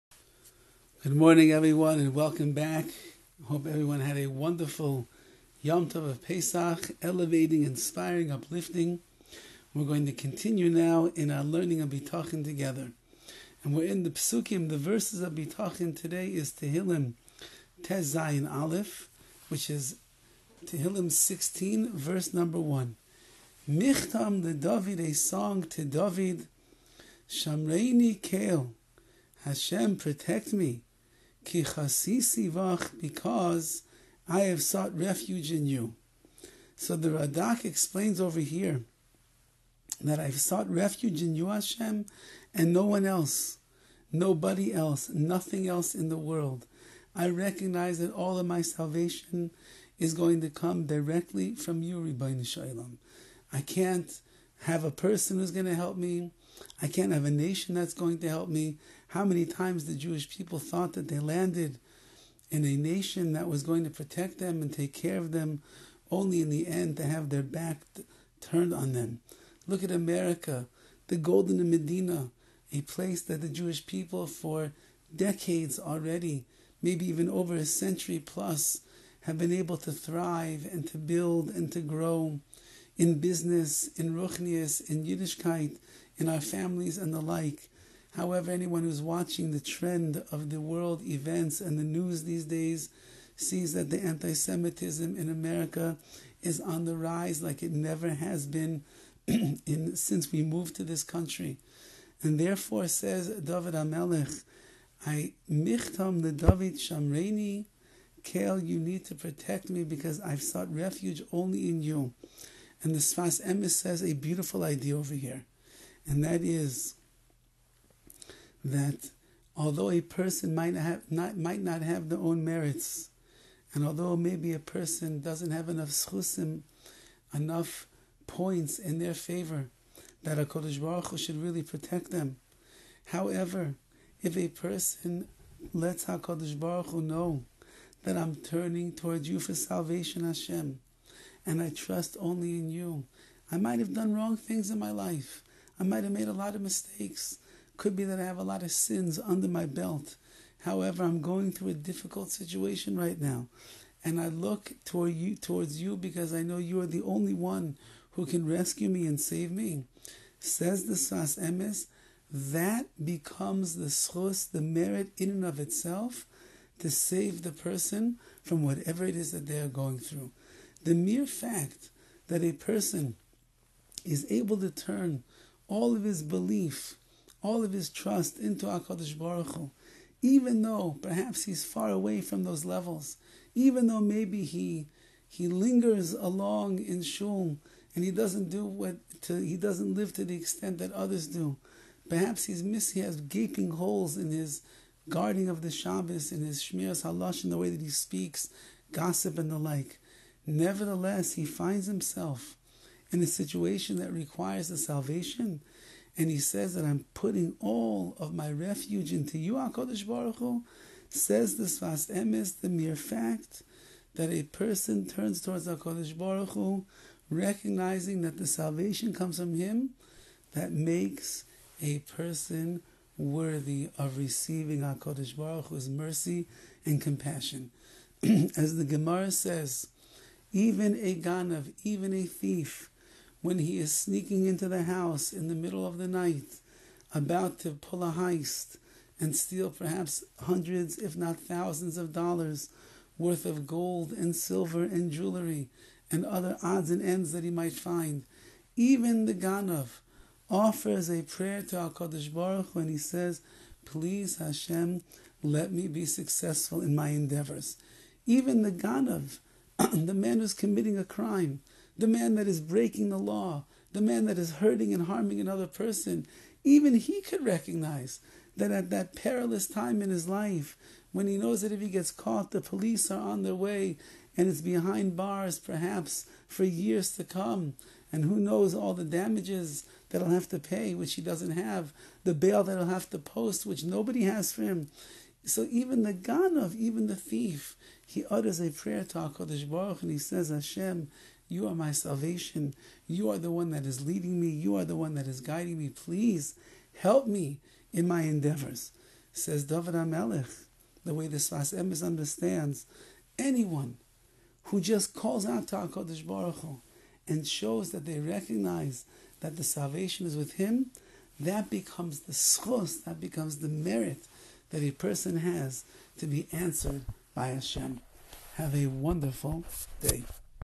Trusting In Hashem Is A Great Merit - Bitachon #151
Bitachon #151
Trusting In Hashem Is A Great Merit
by Rabbi Dovid Horowitrz